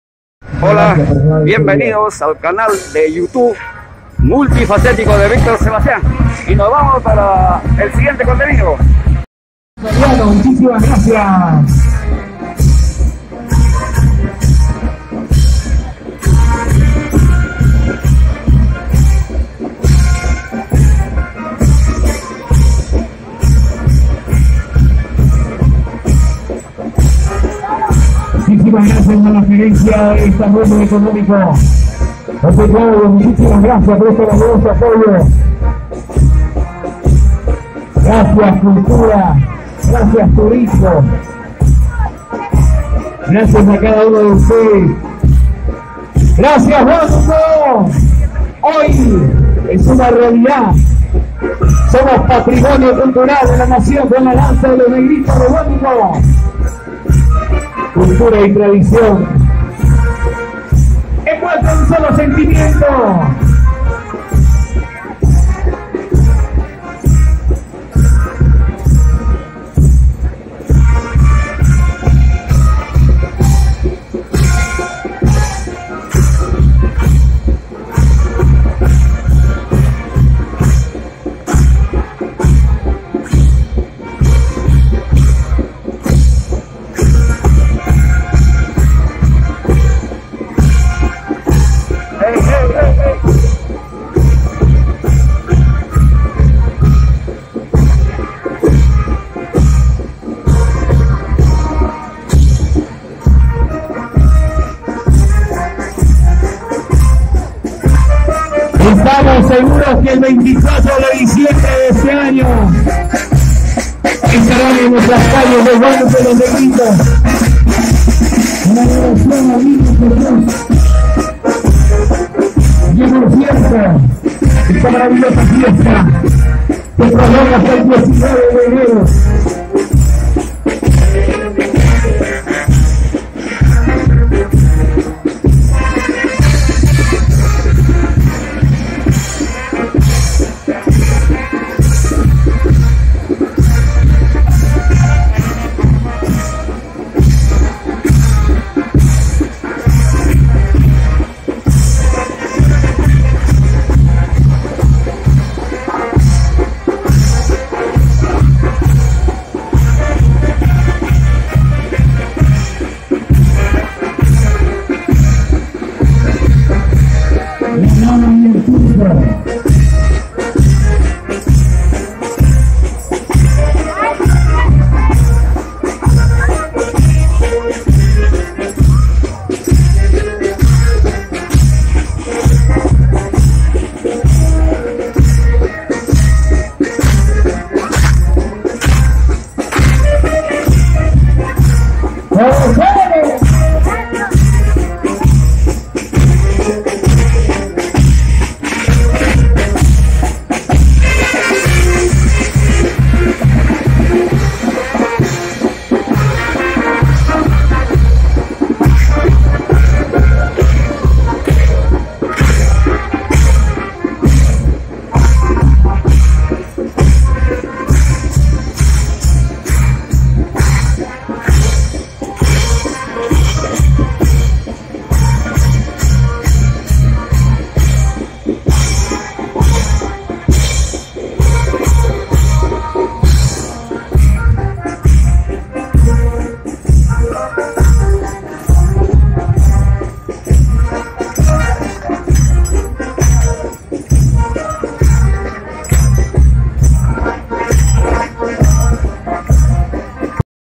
DANZA LOS NEGRITOS DE HUÁNUCO PERÚ patrimonio nacional
DANZA NEGRITOS DE HUÁNUCO PERÚ  PATRIMONIO CULTURAL DE LA NACIÓN PERUANA.
Un día como hoy 7 de julio del 2021 se reconoce como patrimonio cultural de la.
NACIÓN PERUANA A LA DANZA DE LOS NEGRITOS DE HUÁNUCO PERÚ.
VÍCTOR SEBASTIÁN